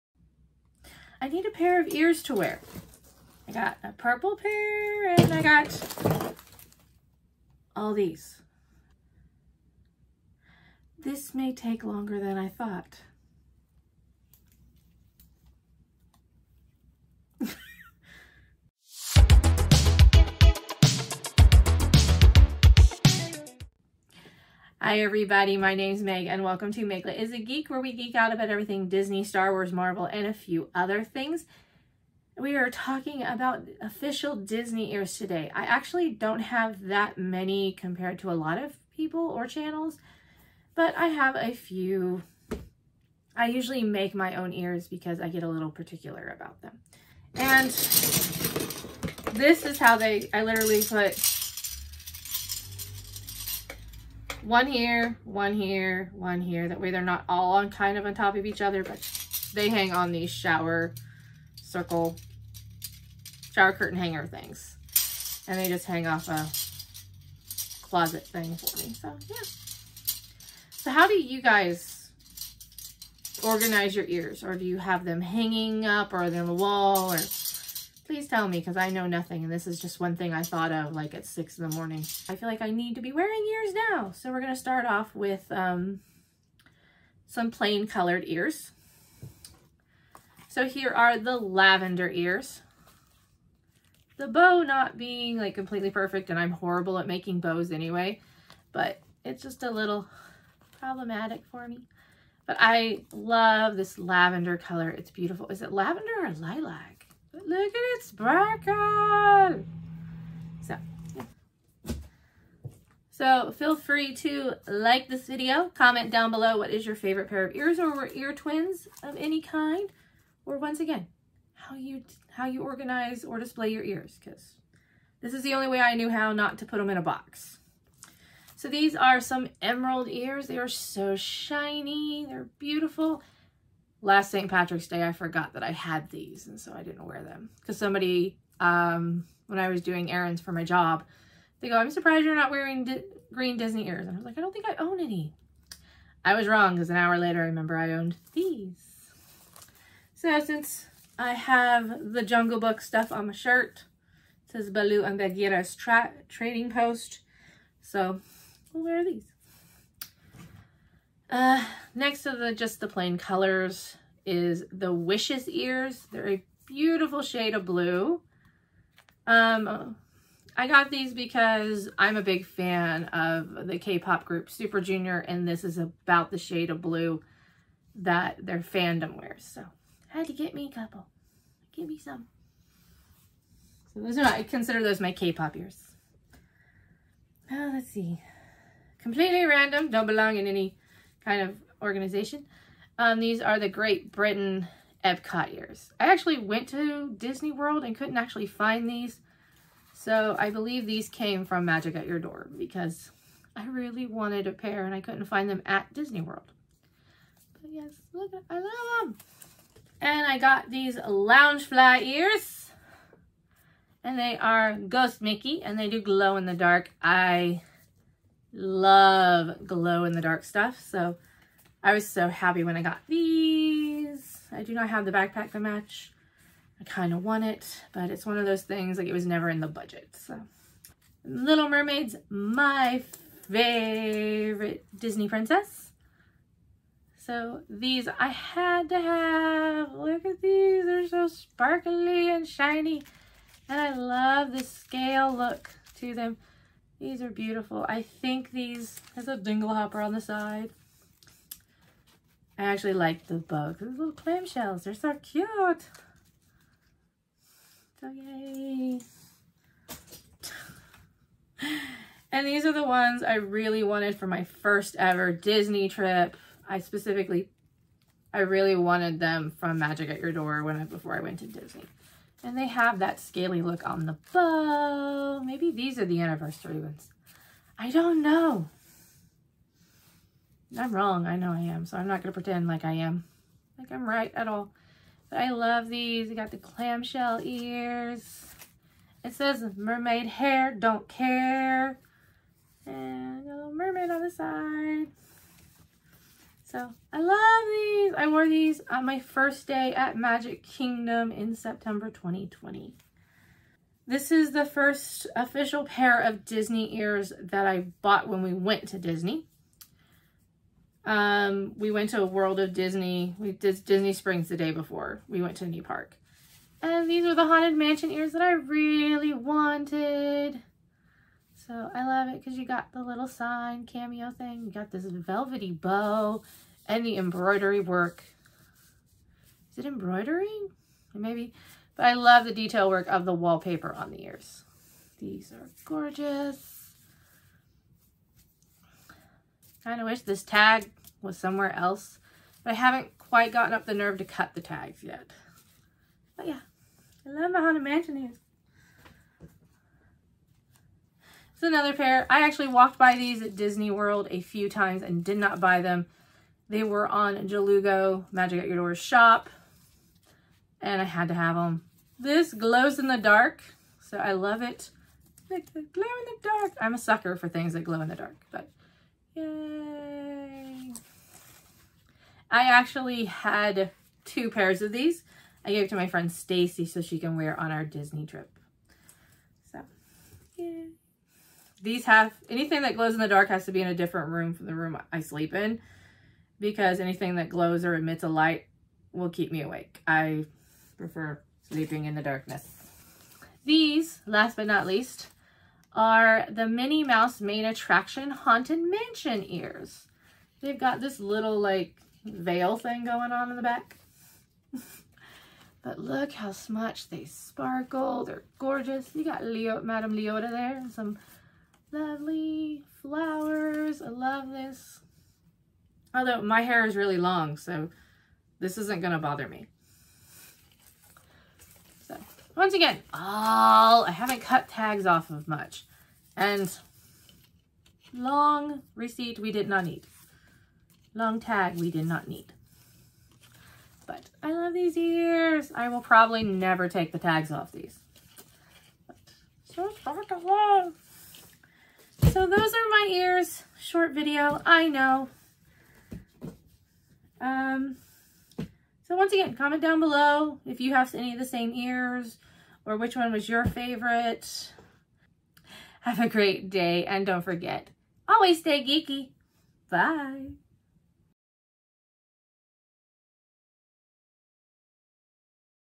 Official Disney Ears Collection
My small Official Disney Ears collection. Basically Haunted Mansion and Little Mermaid.

How do you display your ears???